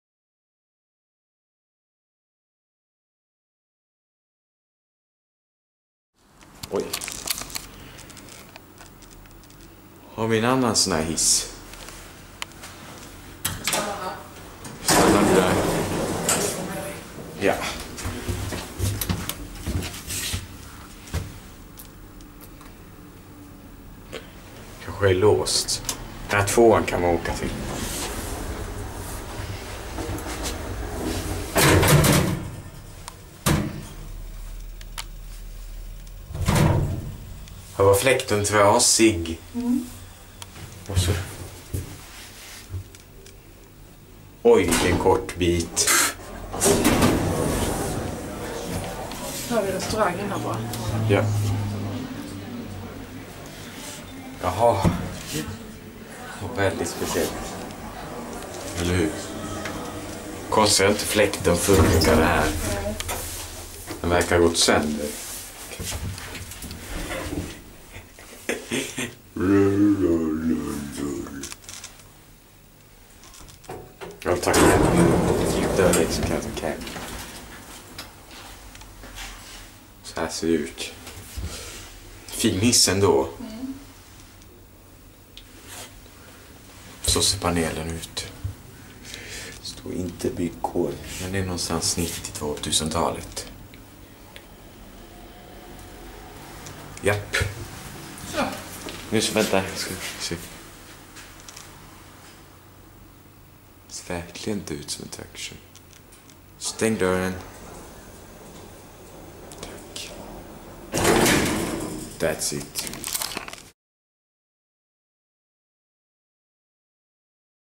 Short ride! H.M. lyftur hydraulic elevator @ Some restaurant house in Reykjavik, Iceland.
Brand: H.M. lyftur
Type: Traction
Capacity: 630 KG / 8 persons
Year installed: Unknown
Floors: 4 (^0^ *1* 2 ^3^)

Recorded: 25/6-2015